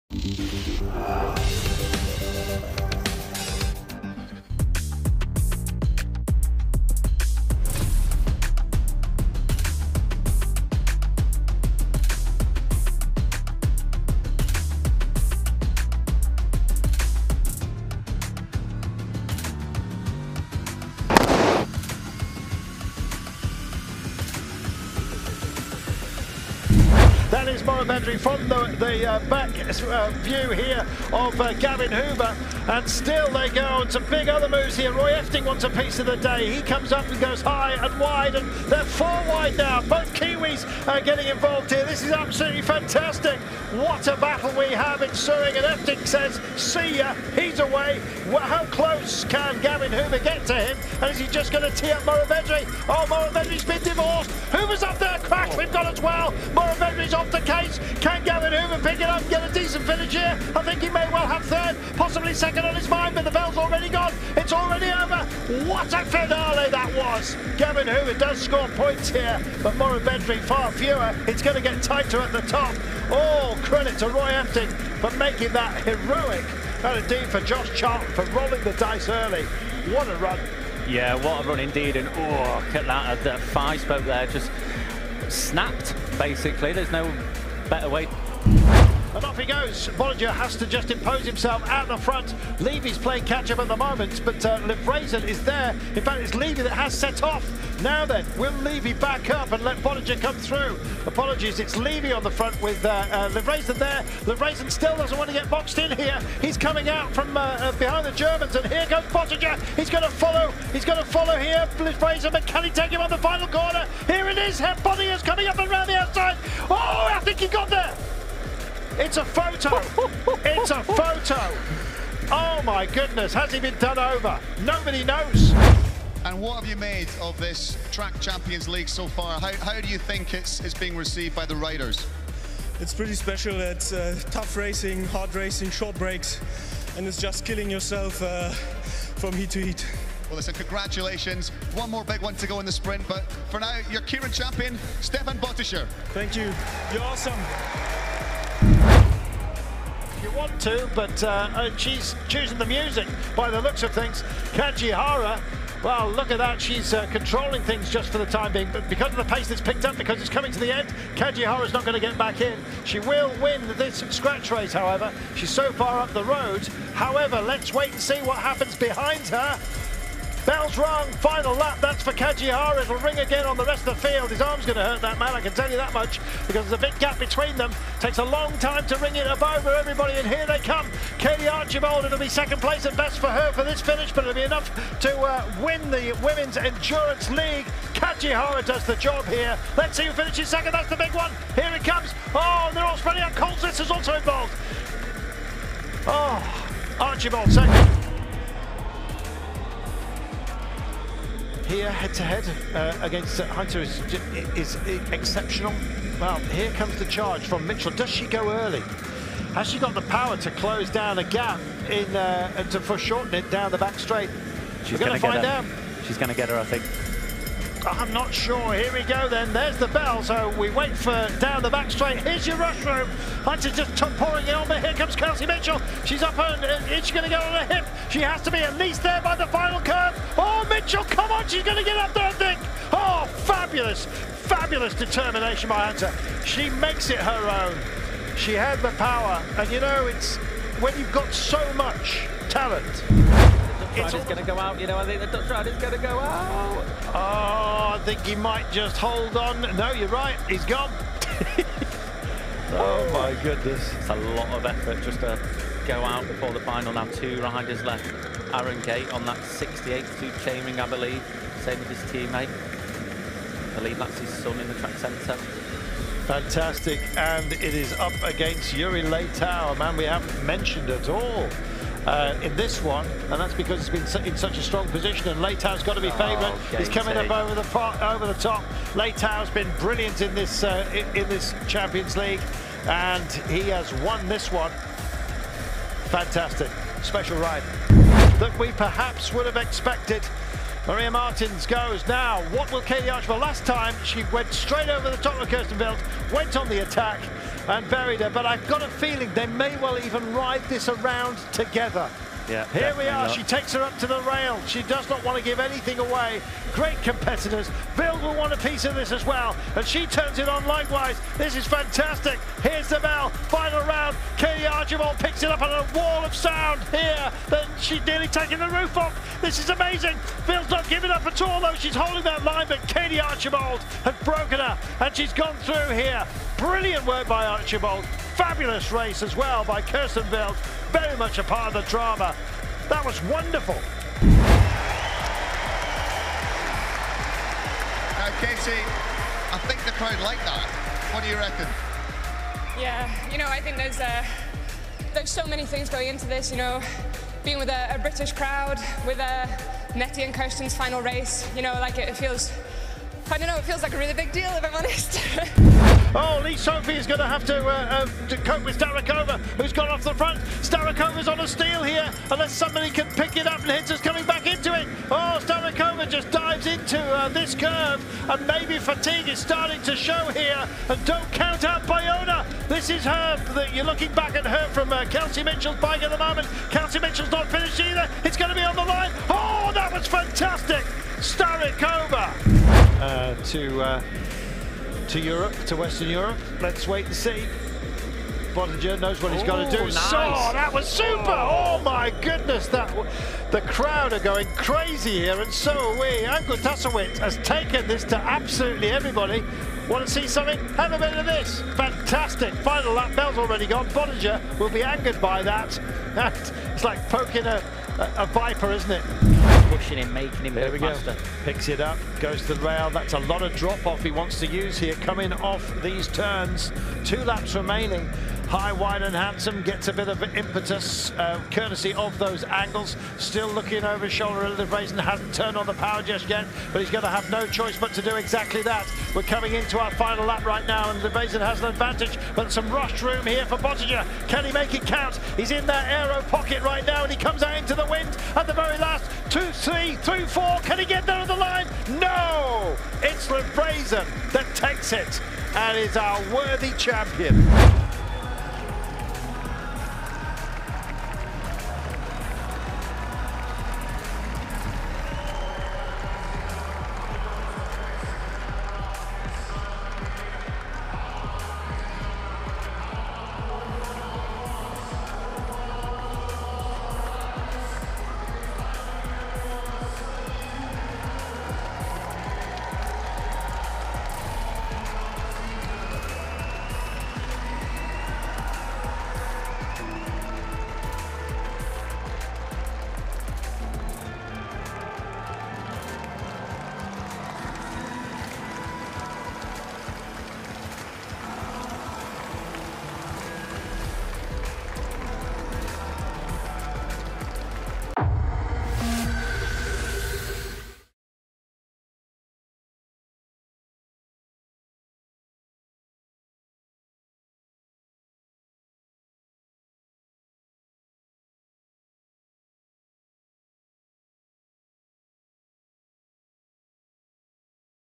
Grand Finale: London - Highlights | UCI Track Champions League | Eurosport
Watch the best moments of Round 4 (Grand finale) of the 2021 UCI Track Champions League in London, UK.